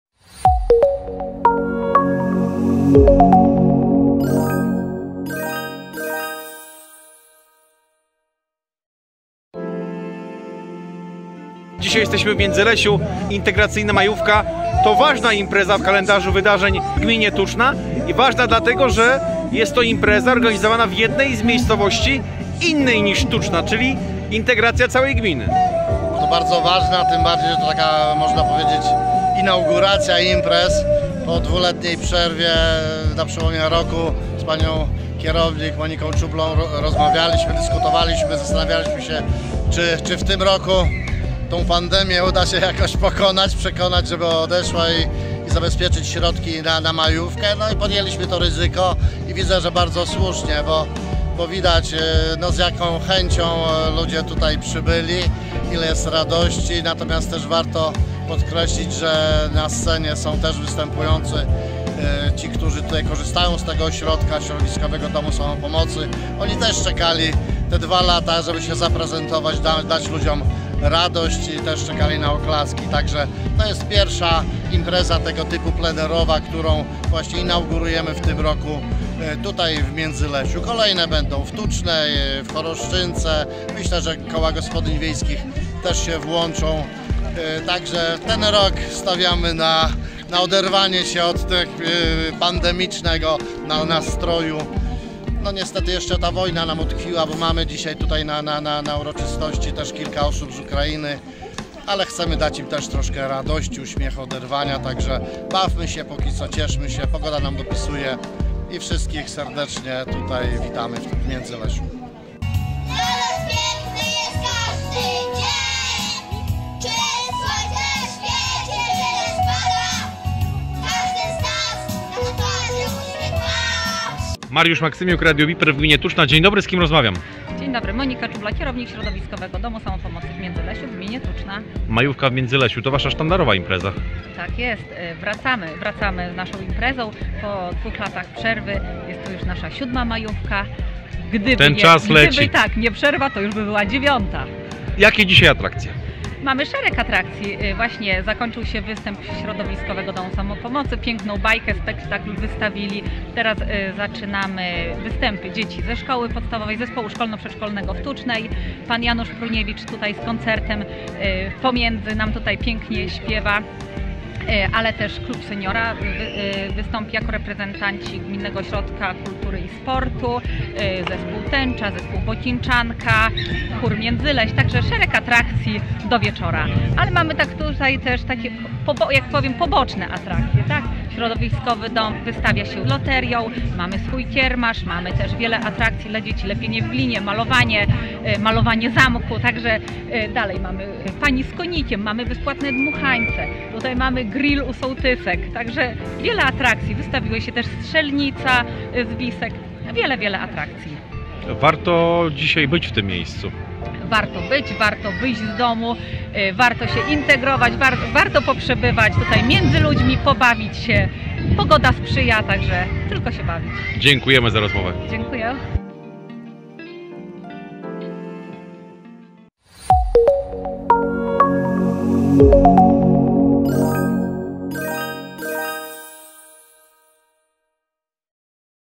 Tuczna: V Majówka Integracyjna w Międzylesiu | Radio Biper
27 maja w Międzylesiu bawiono się się na V Majówce Integracyjnej zorganizowanej przez gminę Tuczna oraz Ośrodek Samopomocy w Międzylesiu.